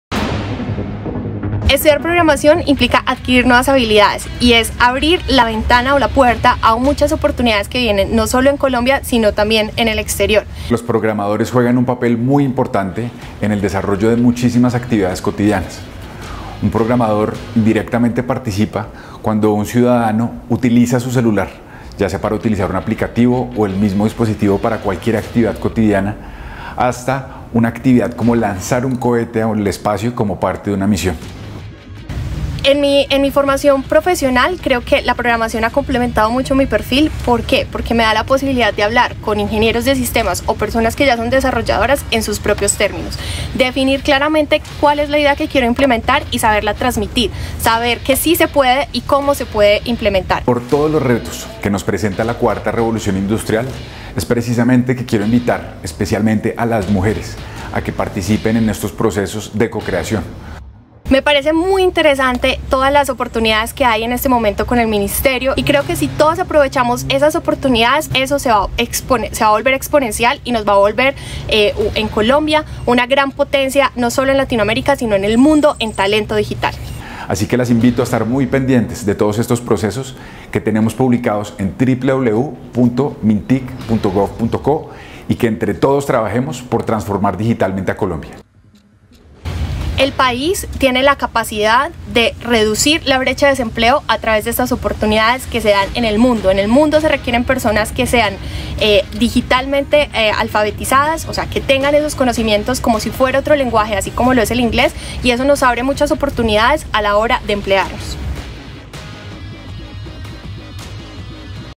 Un homenaje para los programadores de nuestro país
El 12 de septiembre se celebró el día del programador, un oficio que nos ha facilitado la vida en la actualidad; gracias a los programadores, con sus códigos y etiquetas, nos hemos podido interconectar el mundo.